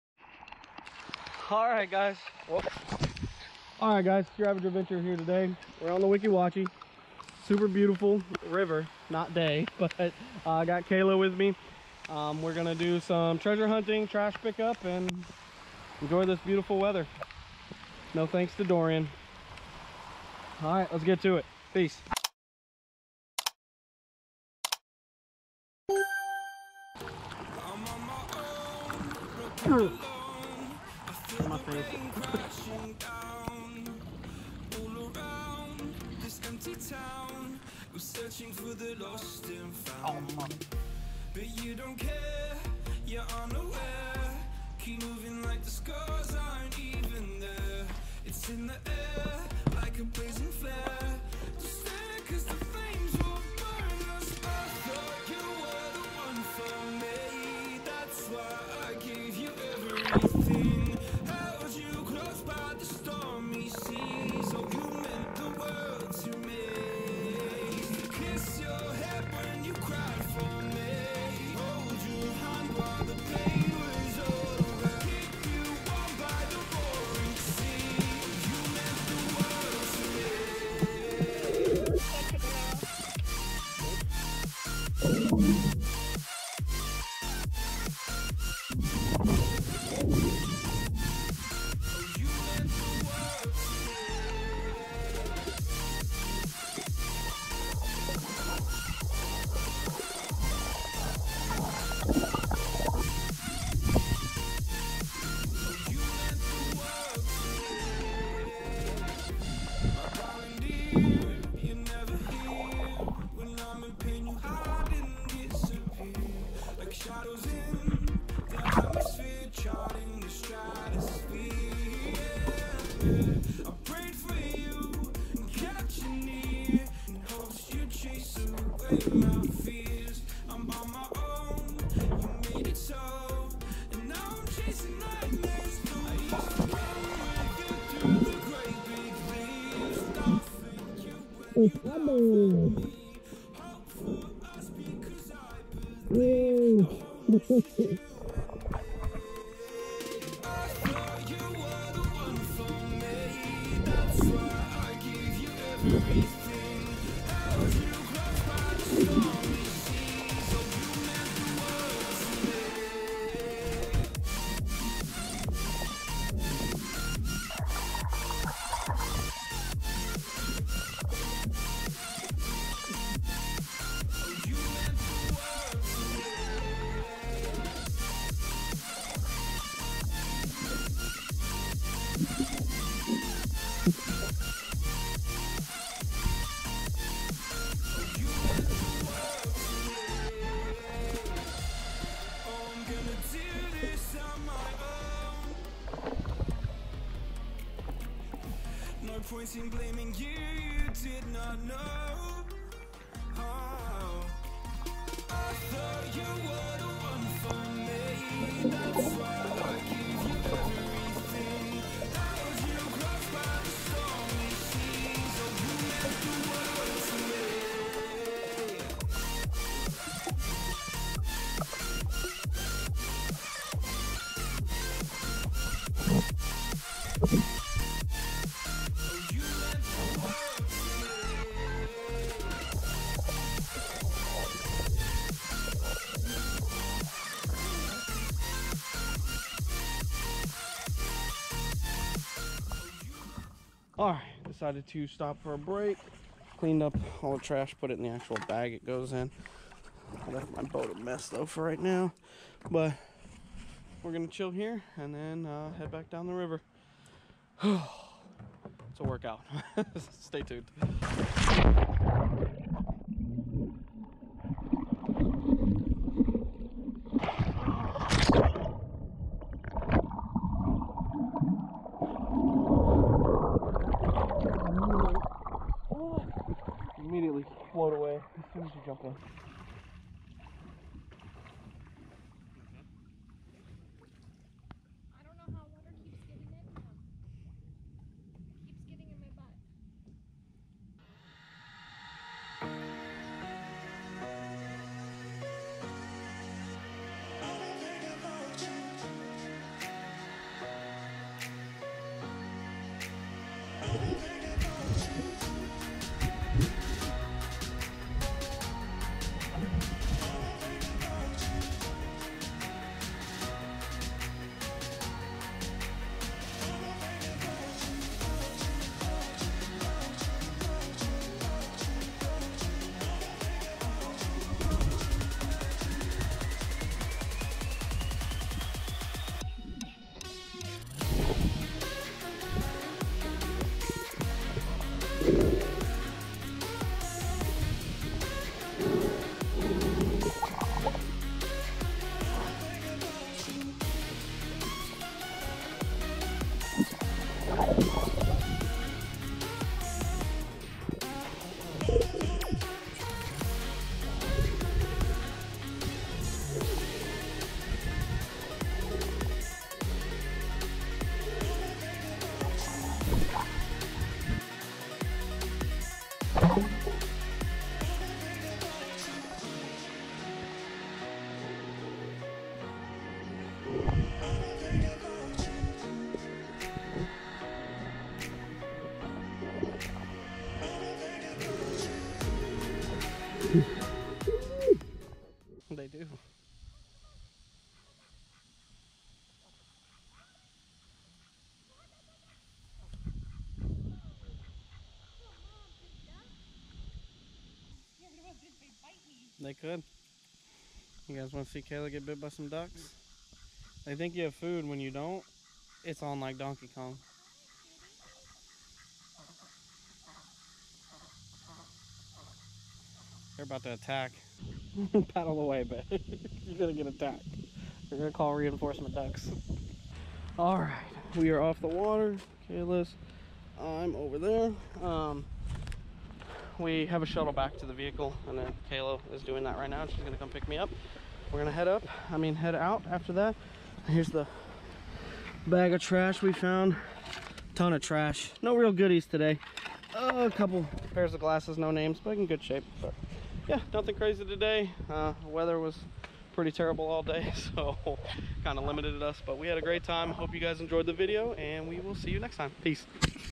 Searching For Trash and Treasure Along Famous Florida River
We had this trip planned a month out, Hurricane Dorian ended up throwing some weather out way tho. We ended up making the best of it and the trip turned out awesome! Hope you all enjoy the video as much as we did making it!!

Da Jams!